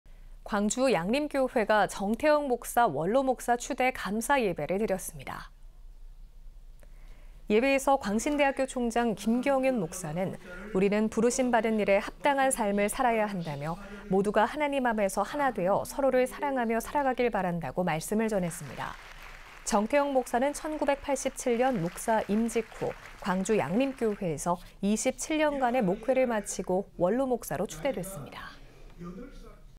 광주양림교회 정태영 목사 원로목사 추대 감사예배 (광주, 이주연)ㅣCTS뉴스
광주양림교회가 정태영 목사 원로목사 추대 감사예배를 드렸습니다.

예배에서 광신대학교 총장 김경윤 목사는 “우리는 부르심 받은 일에 합당한 삶을 살아야 한다”며 “모두가 하나님 안에서 하나 되어 서로를 사랑하며 살아가길 바란다”고 말씀을 전했습니다.

정태영 목사는 1987년 목사 임직 후, 광주양림교회에서 27년간의 목회를 마치고 원로목사로 추대됐습니다.